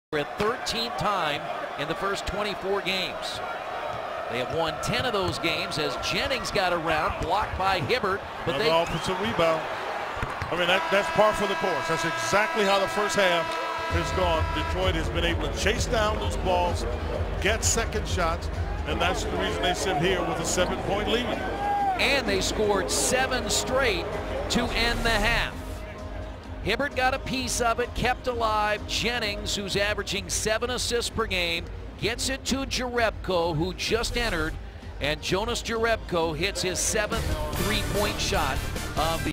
Jonas Jerebko Hits the Three Point Shot Buzzer Beater
Indiana Pacers  Vs Detroit Pistons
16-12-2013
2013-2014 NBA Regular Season